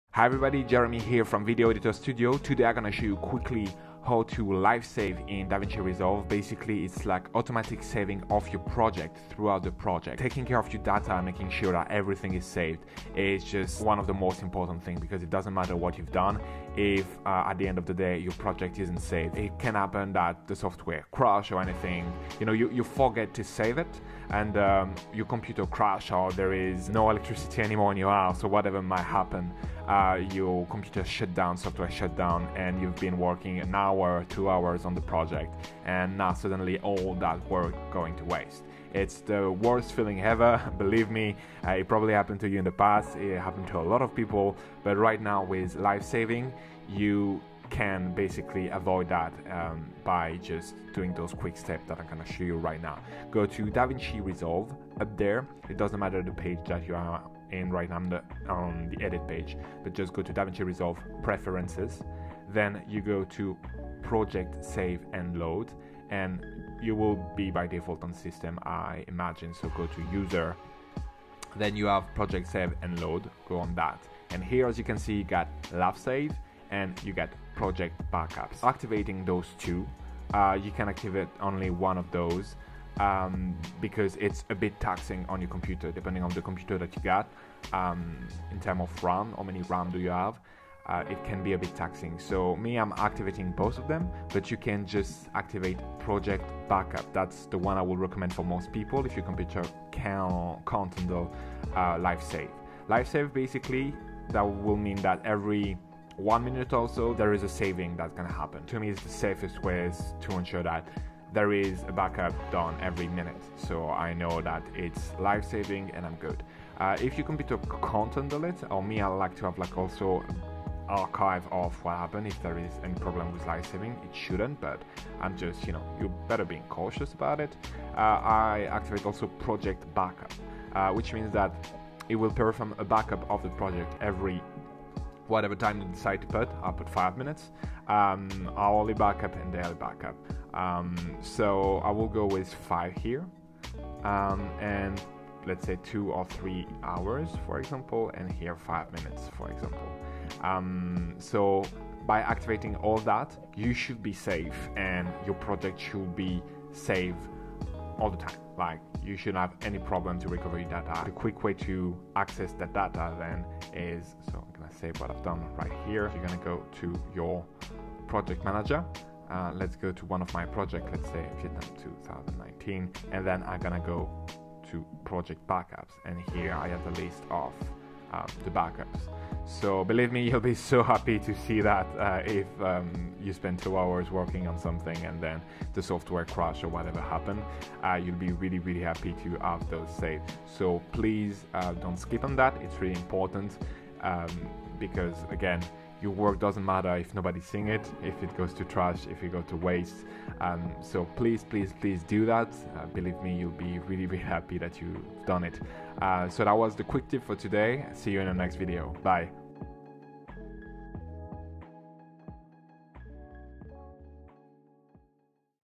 How to Auto Save and Backup projects in Davinci Resolve
In this video, I'll explain to what I think is one of the most important functionality to enable in Davinci Resolve. It's the Live Save and Auto Backup in Davinci, there's nothing worst than working hours on a project and suddenly the software or your computer crash and you didn't save it. With this functionality, it will never happen ever again. All your projects will be saved!

Let me know what video you would like to see next!